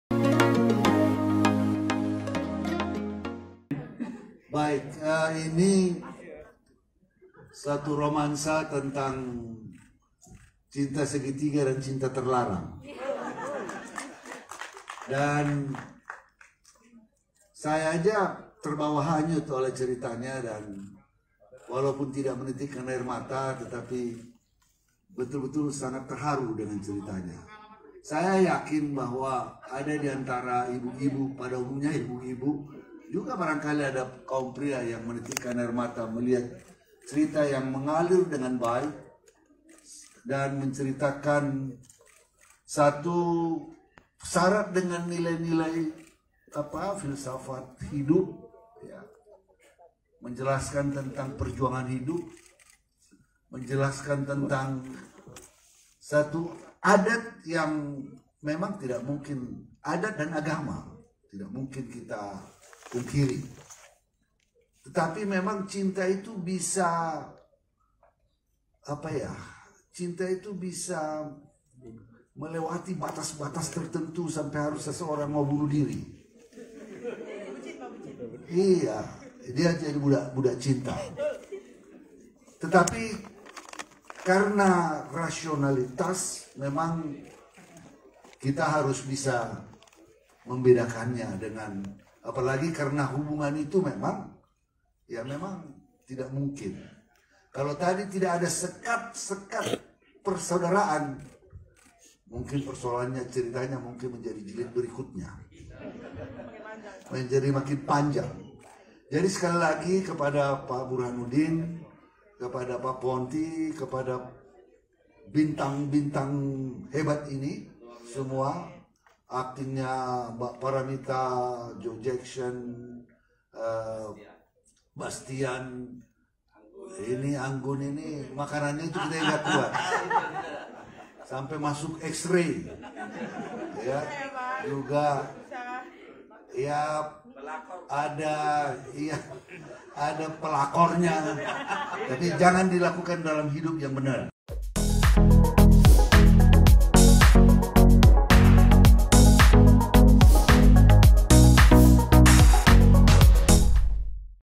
Menkumham Yasonna Laoly Terharu Nonton FIlm Nariti
Grid.ID - Menkumham Yasonna Laoly Terharu Nonton FIlm Nariti.
Tak hanya mengangkat keindahan alam Kawasan Danau Toba, melalui garapan Film Romansa Danau Toba diharapkan dapat menjadi momentum kebangkitan bagi para aktor dan aktris asal Sumut dikancah perfileman di tanah air.
Oleh karenanya Eksekutif Produser film Romansa Danau Toba, Burhanuddin tak segan-segan menyampaikan kalau Romansa Danau Toba nantinya akan diisi talenta-talenta baru dari Sumut.

EDITORIAL OFFICE

ADVERTISING